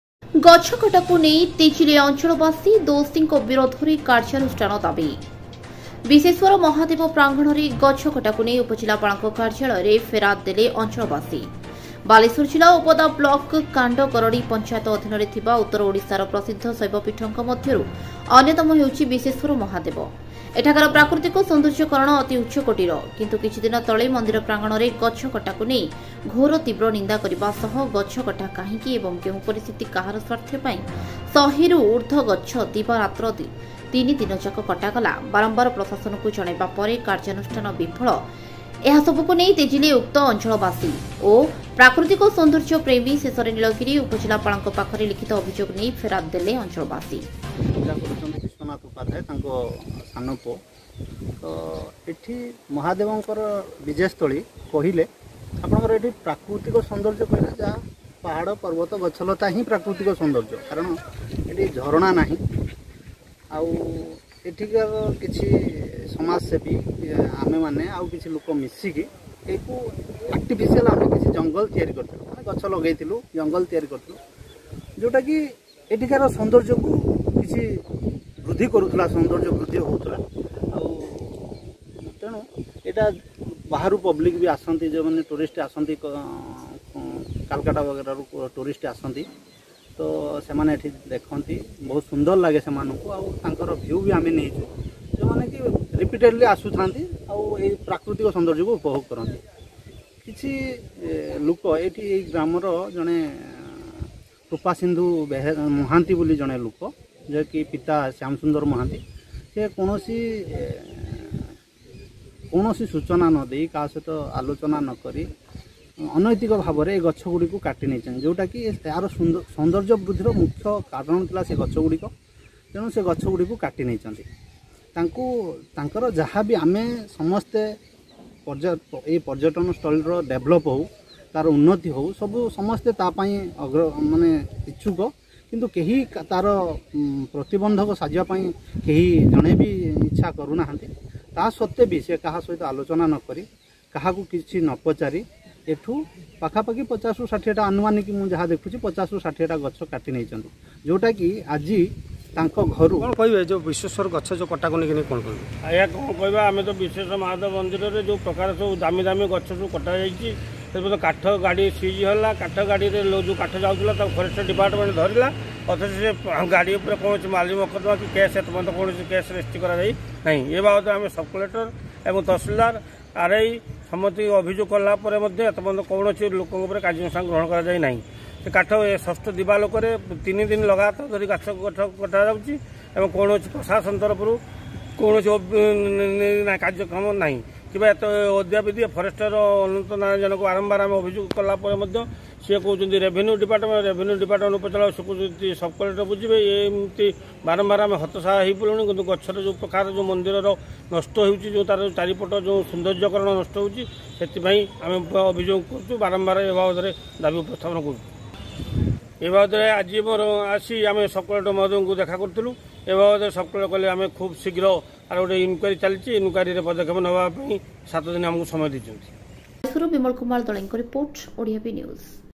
ଗଛ କଟାକୁ ନେଇ ତେଜିଲେ ଅଂଚଳ ବାସୀ,ଦୋଷୀ ଙ୍କ ବିରୋଧ ରେ କାର୍ଯ୍ୟାନୁଷ୍ଠାନ ଦାବି!!Odia B News!!
ଗଛ କଟାକୁ ନେଇ ତେଜିଲେ ଅଂଚଳ ବାସୀ,ଦୋଷୀ ଙ୍କ ବିରୋଧ ରେ କାର୍ଯ୍ୟାନୁଷ୍ଠାନ ଦାବି,,,